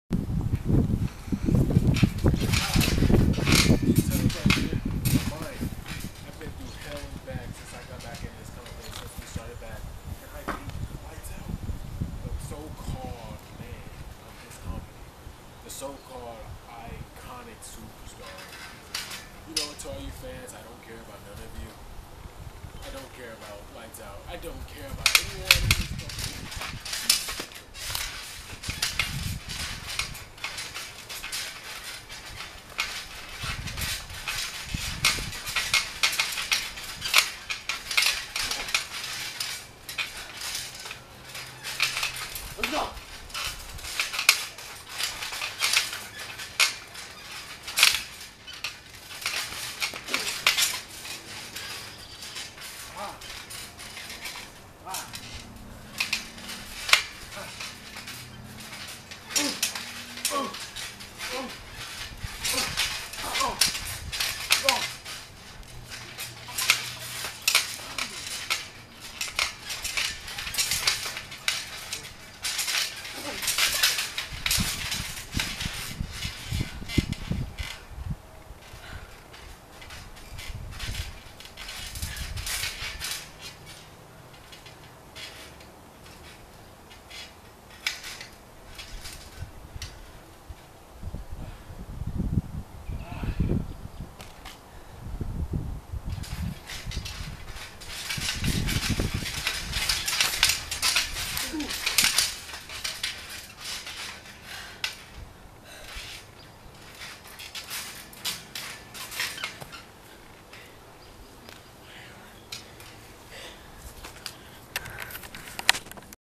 Wwt Wrestling Things Are Personal
This Season Is Heating Up We Have New Talent Coming In And We Have A Lot Of Surprises But This Rivalry Between Moneymaker And Lights Out Its Obvious That Things Are Personal